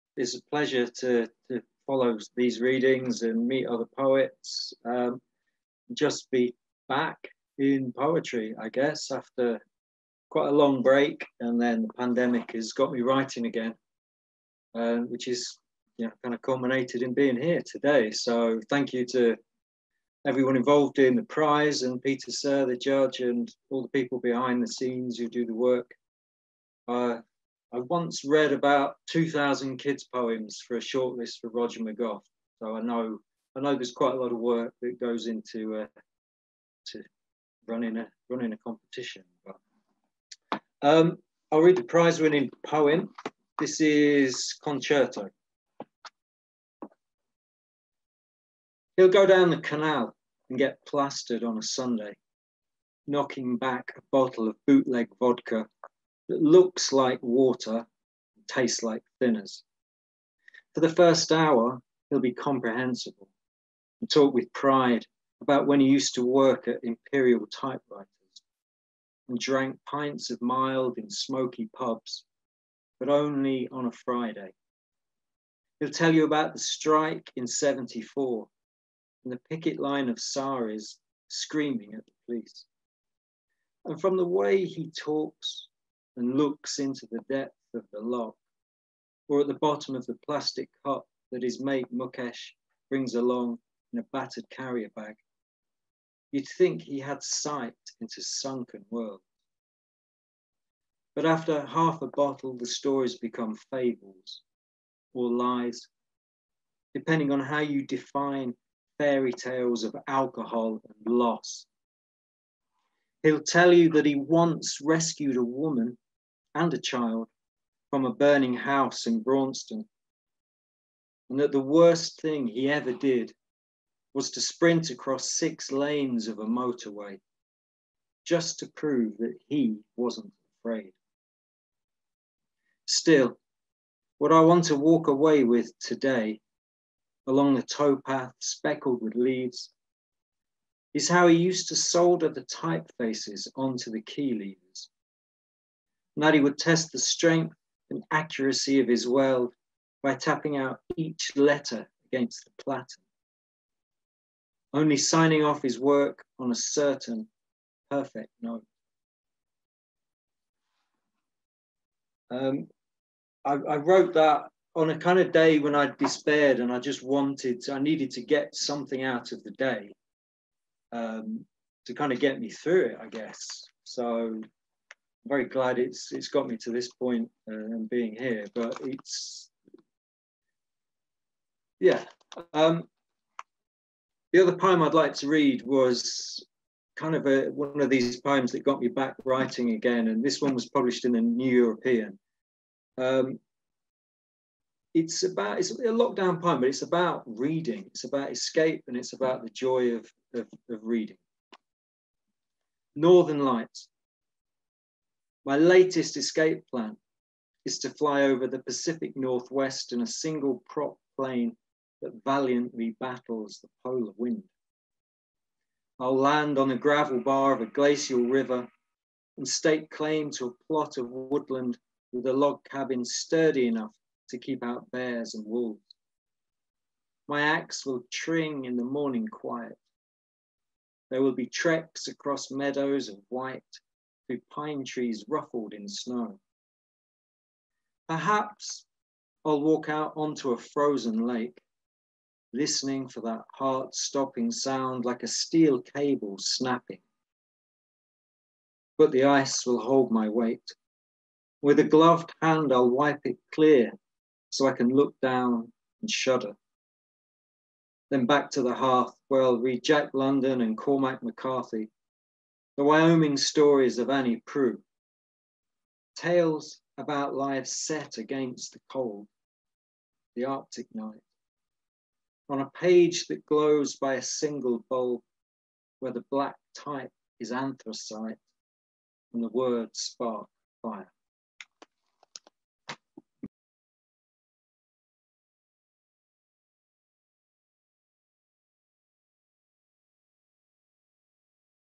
Nicholas Hogg – Gregory O'Donoghue Prize Reading
Nicholas Hogg, first-prize winner of the 2021 Gregory O’Donoghue International Poetry Competition, reading the winning poem “Concerto,” along with several more of his poems at the Cork International Poetry Festival 2021.

Read the winning and shortlisted poems in issue 40 of Southword

Video production: James O'Leary